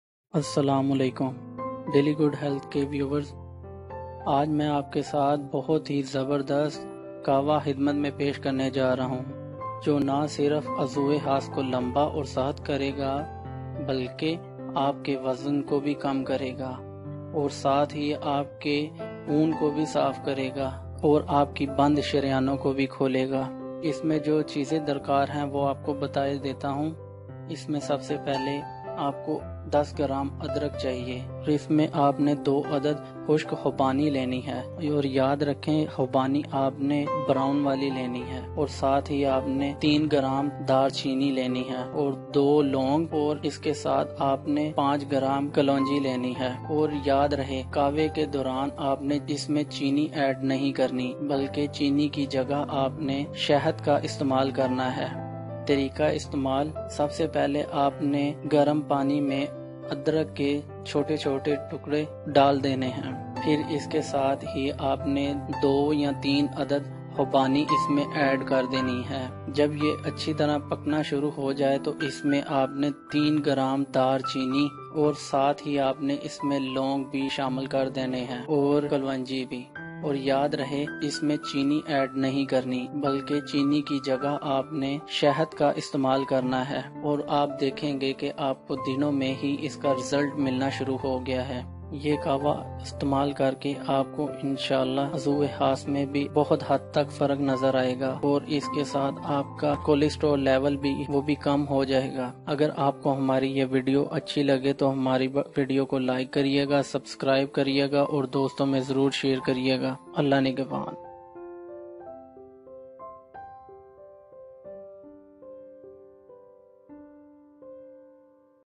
Benefits Of Delicious Green Tea For Weight Loss, Skin & Hair
Green tea is good for hair and skin.
If you are looking for a natural remedy for dry skin and hair, green tea is the answer. It can reduce excessive inflammation and burning on the skin and scalp. Apply tea bags to your face regularly, following hygiene routines. This will help to remove dry spots and redness.